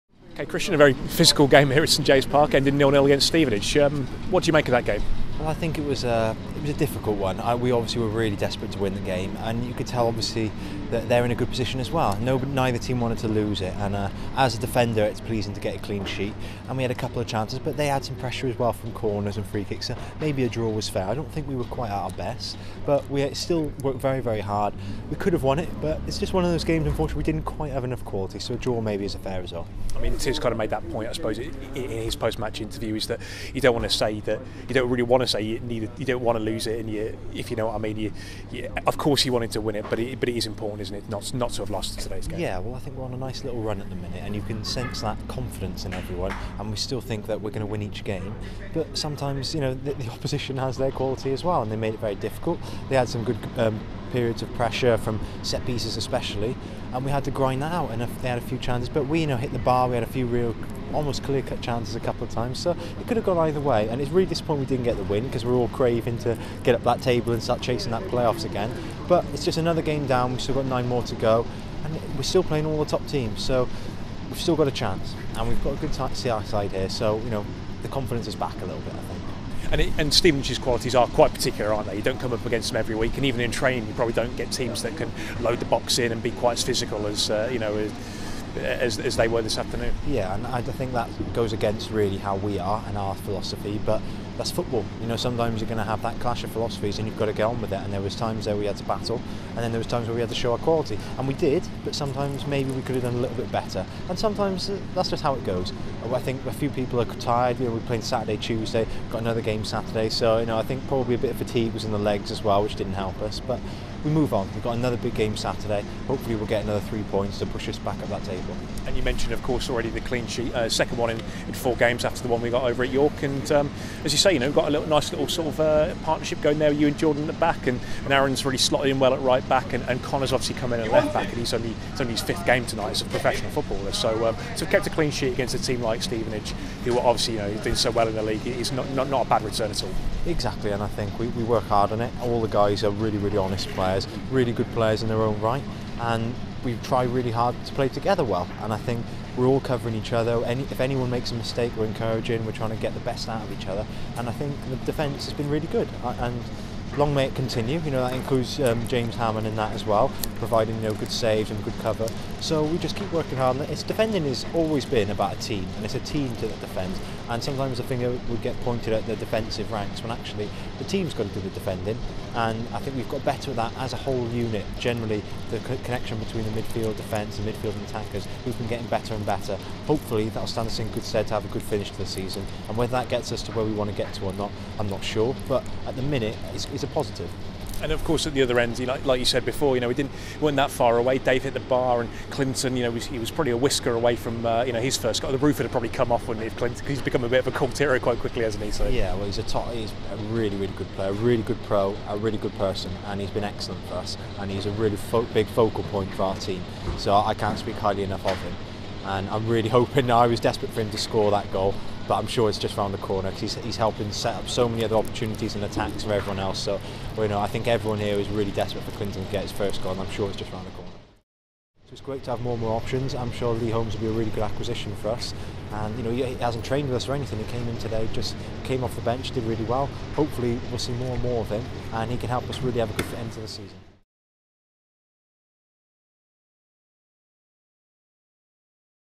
Christian Ribeiro reflects on Stevenage stalemate at the Park | Exeter City Football Club
Exeter City man Christian Ribeiro reflects on a 0-0 with Stevenage at the Park under the floodlights.